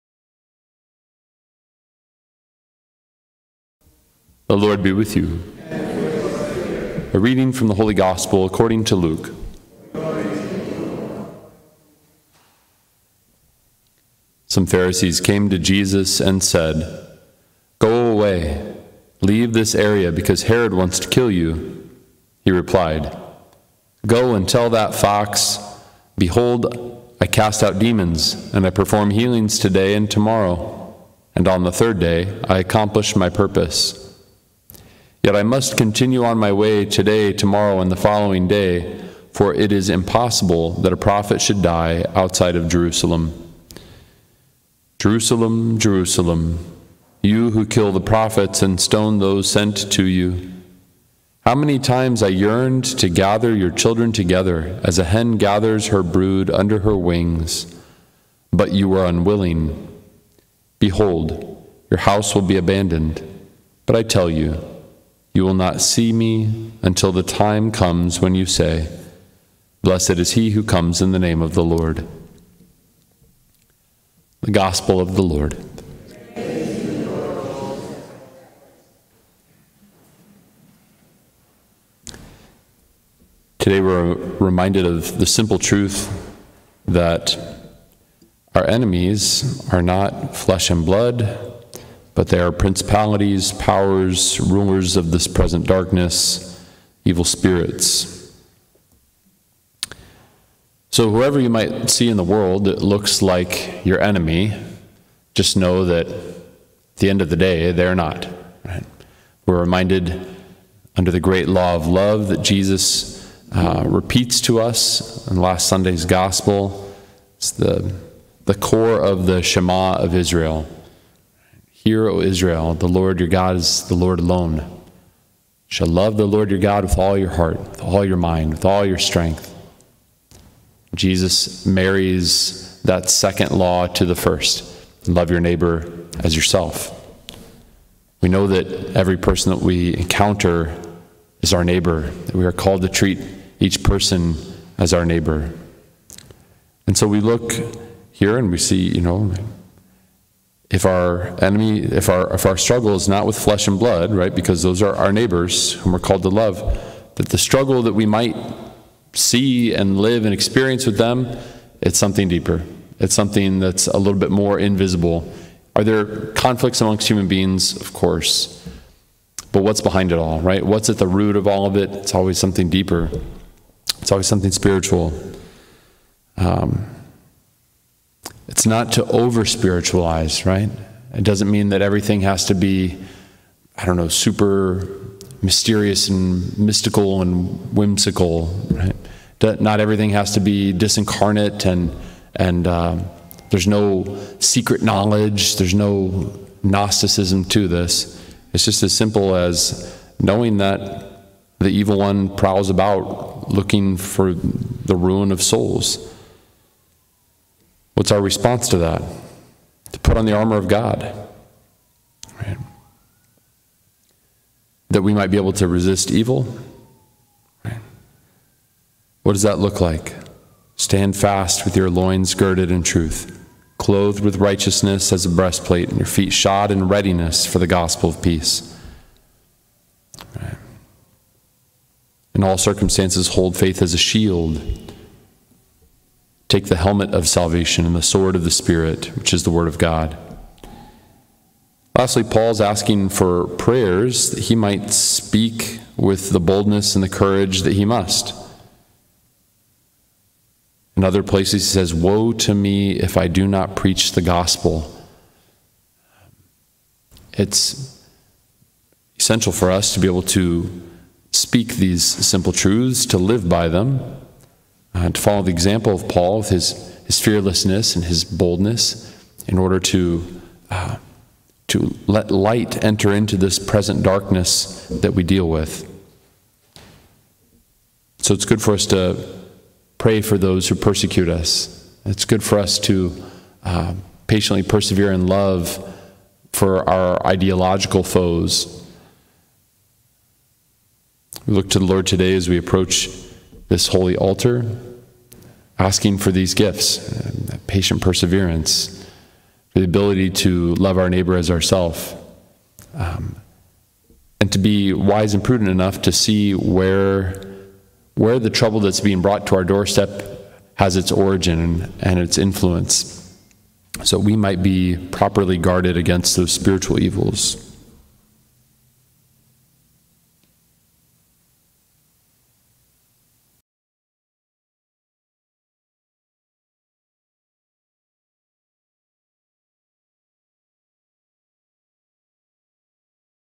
Homily | Our most dangerous enemy is invisible – Fr. Benjamin, 10-29-20
Father Benjamin Lehnertz Homily
“Our most dangerous enemy is invisible"